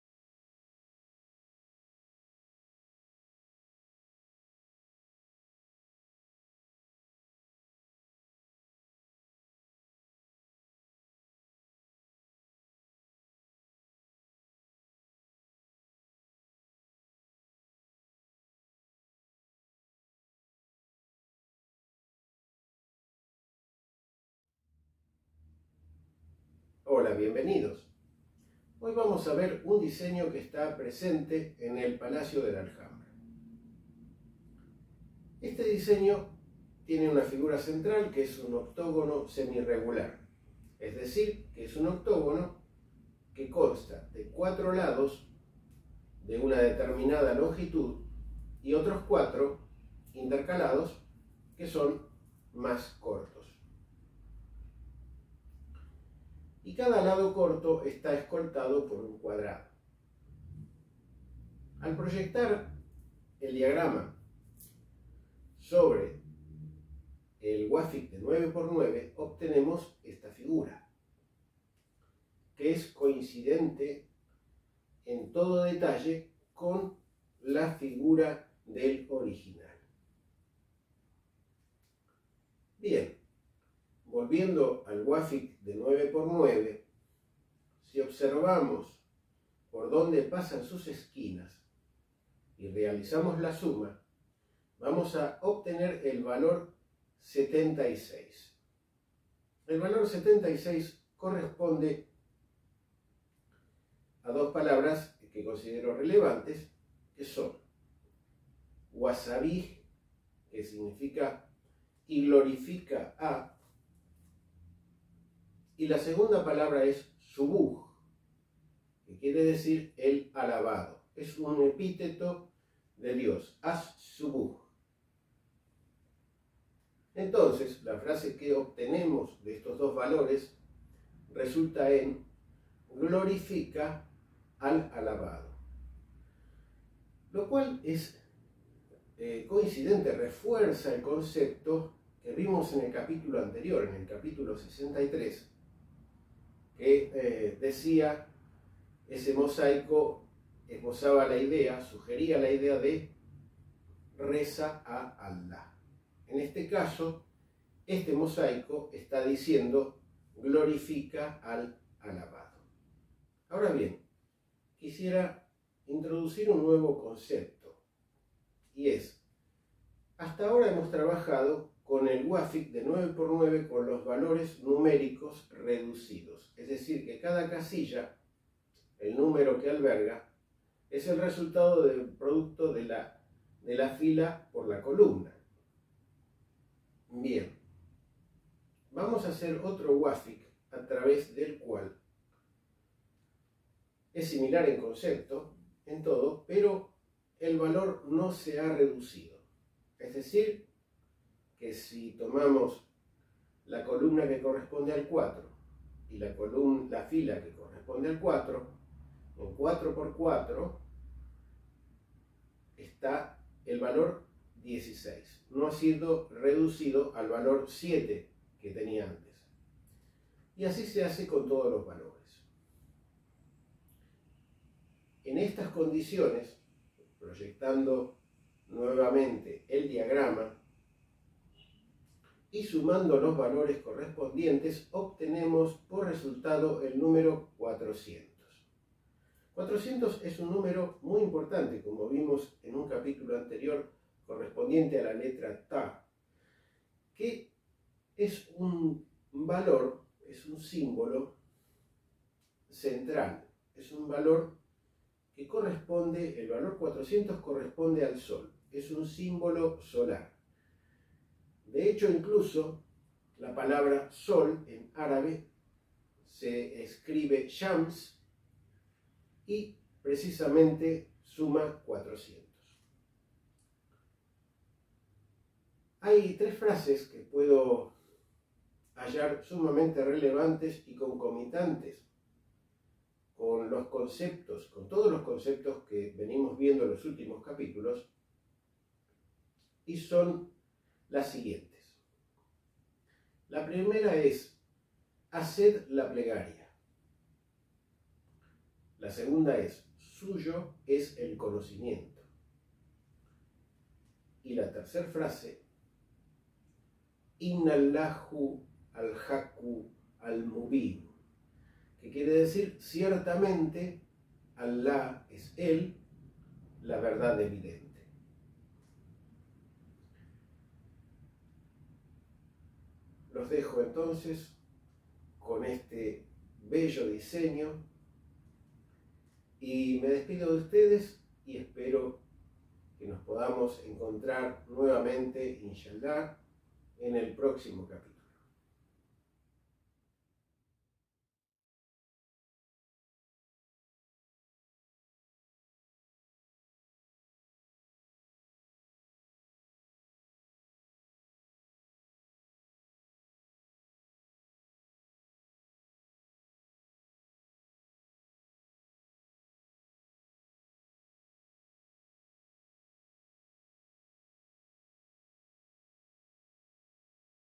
CAP 64 - Wafiq 9x9 - Séptima Parte - Seventh Part
Continuamos con el diseño sobre el Wafiq de 9x9. Si está interesado en Ilm al Huruf, lo invito a la página de FB
We continue with the design on the 9x9 Wafiq. If you are interested in Ilm al Huruf, I invite you to the FB